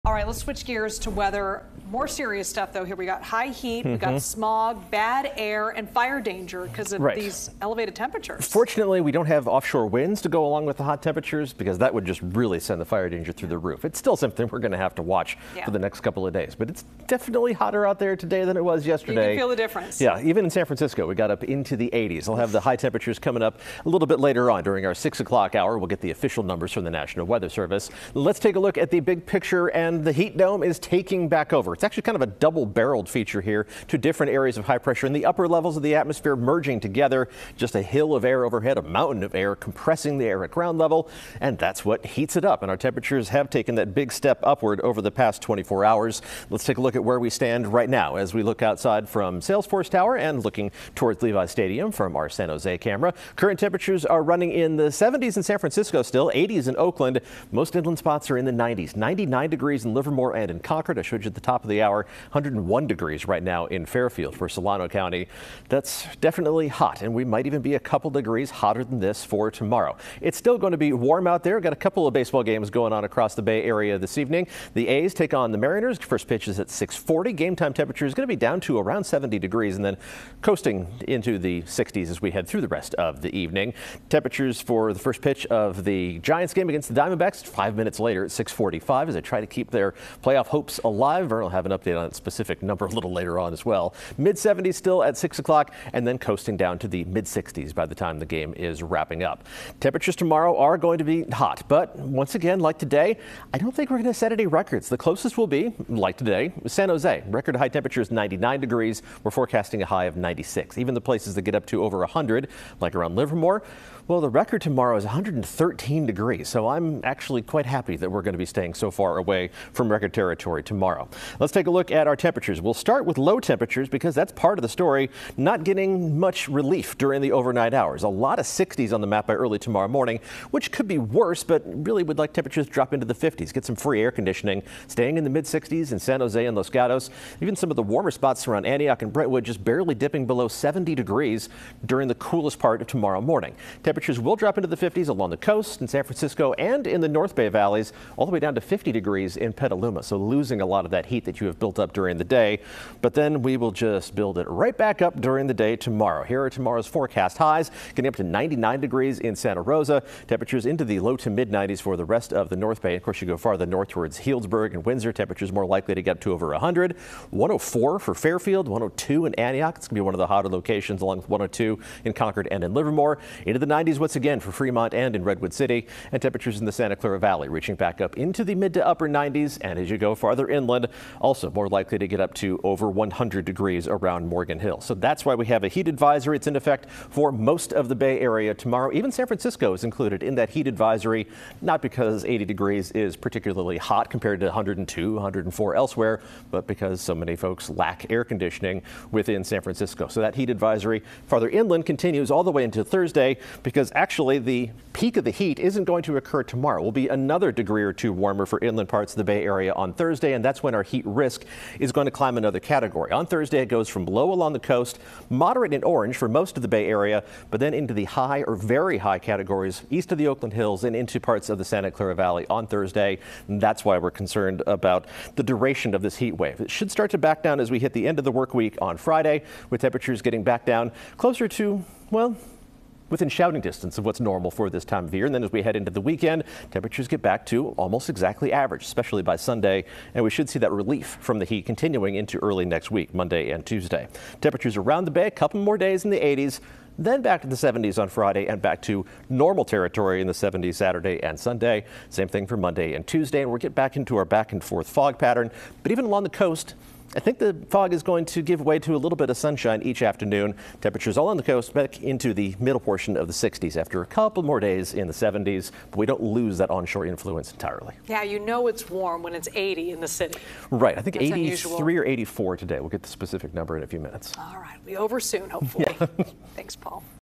Tuesday evening First Alert weather forecast with Paul Heggen - 9/3/24
Expect the heat wave to continue mid-week, with inland highs over 100 on Wednesday. Air quality is also a concern, with a Spare the Air advisory remaining in effect. Paul Heggen has the forecast.